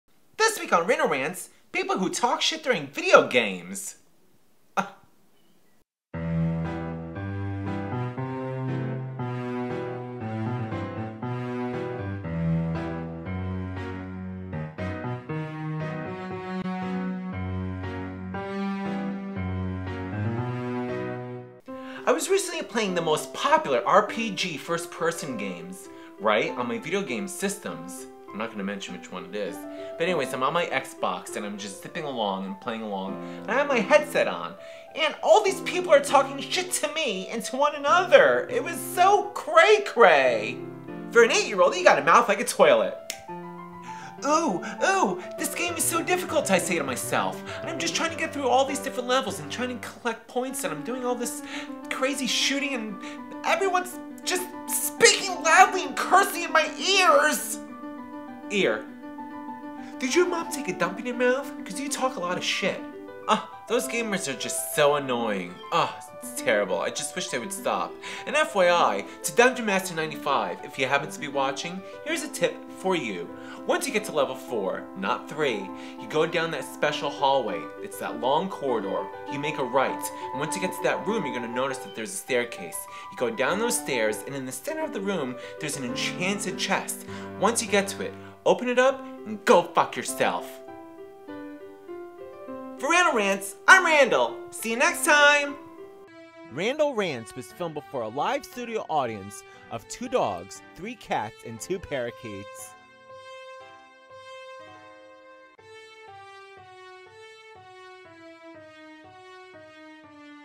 Honey Badger's Randall: Randall Rants - Trash-Talkin' Gamers
Randall Rants about talky gamers.
Randall Rants about a variety of topics and issues. His views and opinions are not to be taken seriously.
for the honey badger app